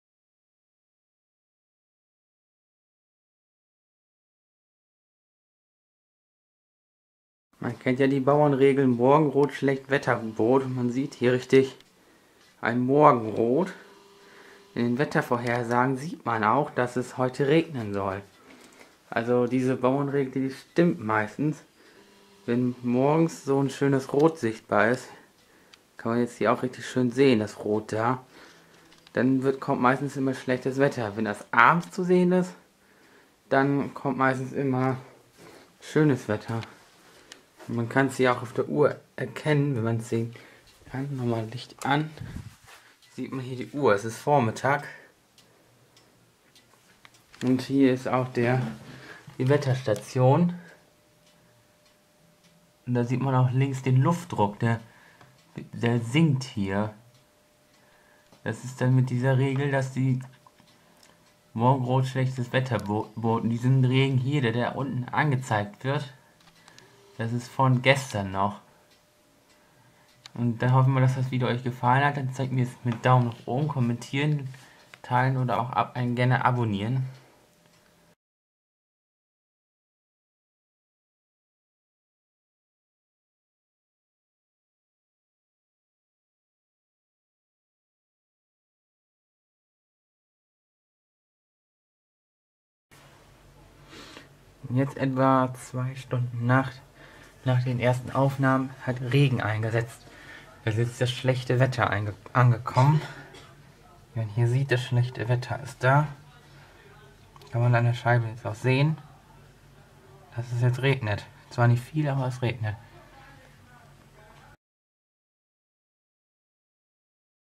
Morgenrot Schlechtwetterbot!
Diese Wetterregel stimmt fast immer (so wie am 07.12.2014)